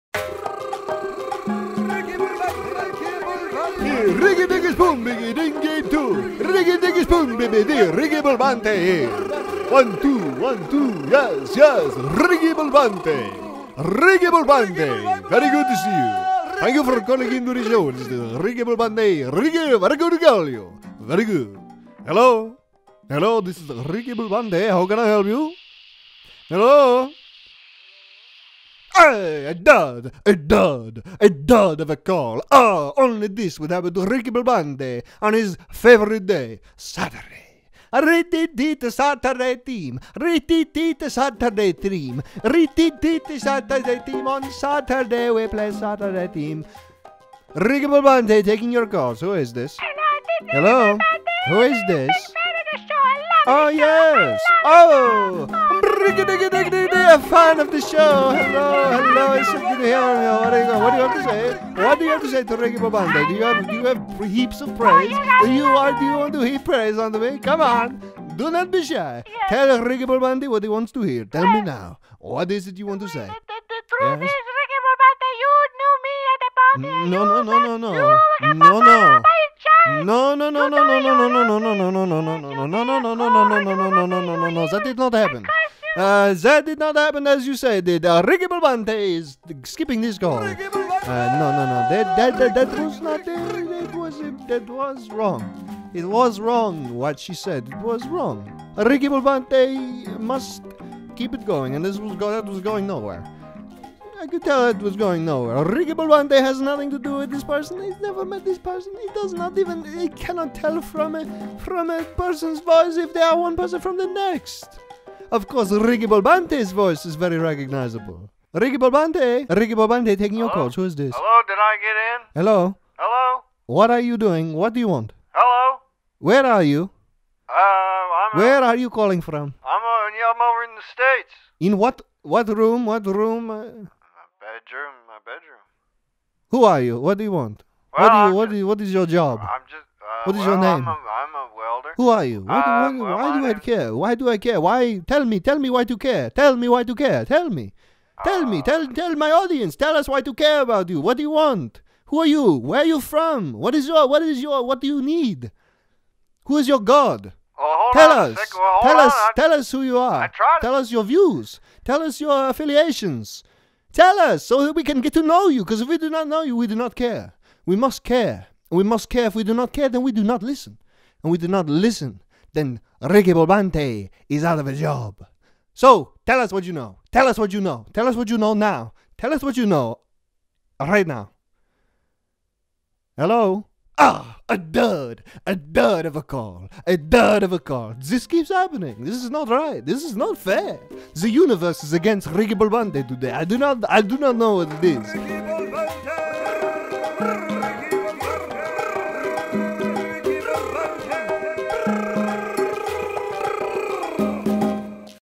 "Very Good To Call You" with Ricky Volbonte
Ricky Volbonte is a man of indeterminate ethnic origin who hosts a radio call in show.